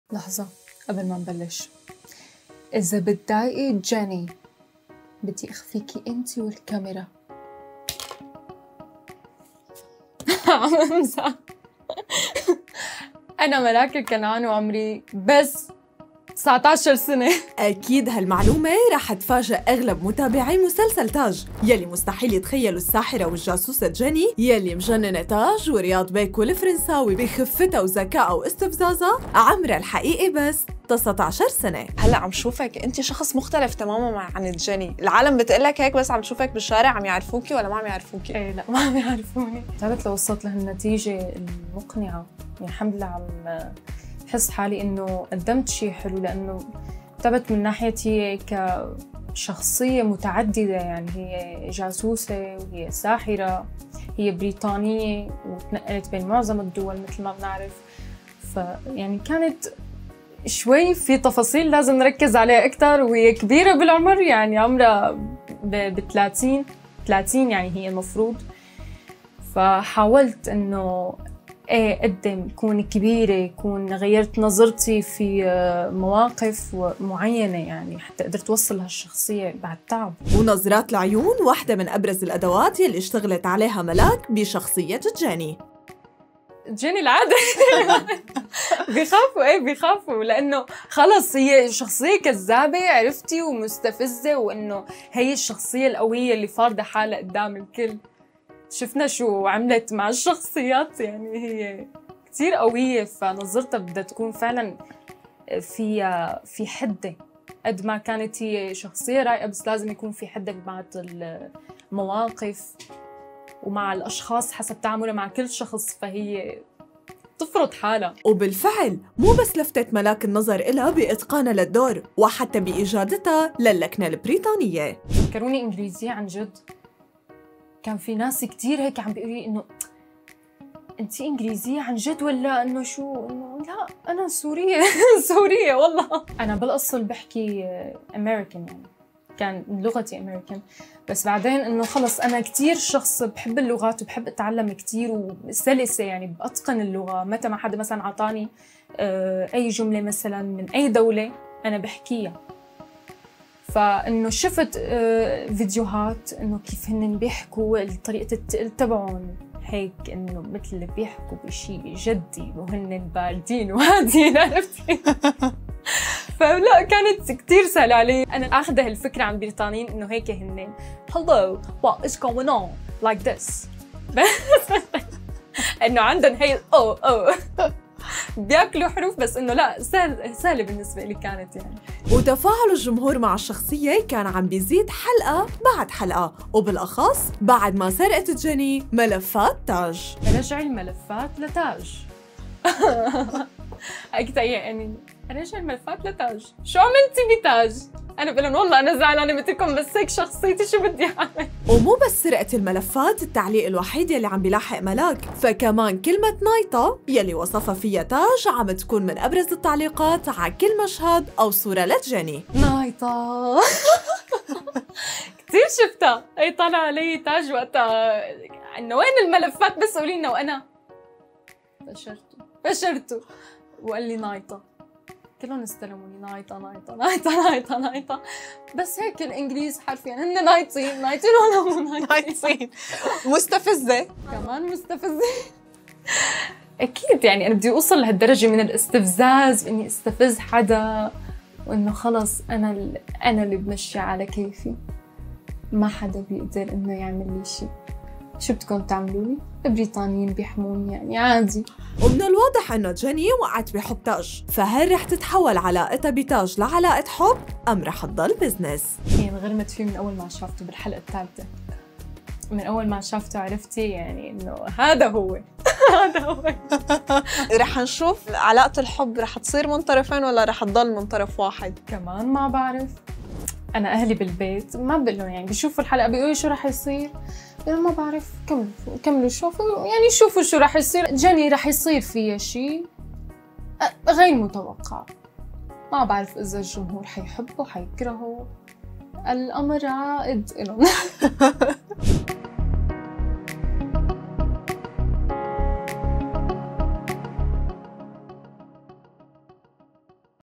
الساحرة "جيني" بأول ظهور إعلامي بعد سرقة ملفات "تاج"
الساحرة "جيني" بأول ظهور إعلامي بعد سرقة ملفات "تاج".. عمرها وشكلها الحقيقي رح يصدمكم!